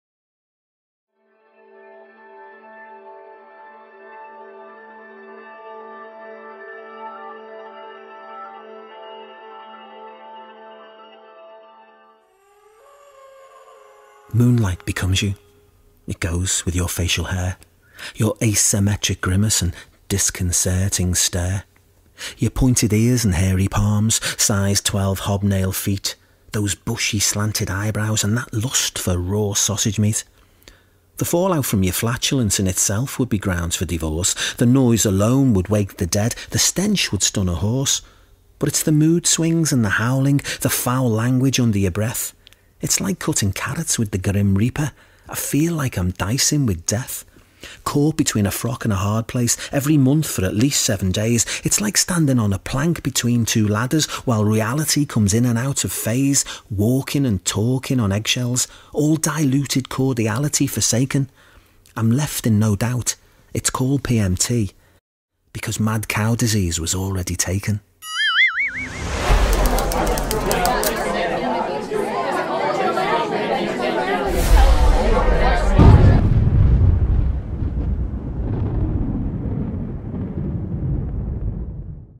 Keith Wilson Poetry : MOONLIGHT
Spoken Word from Liverpool Musician and Poet Keith Wilson
Full-time member of The Spiritualists : Part-time member of The Scaffold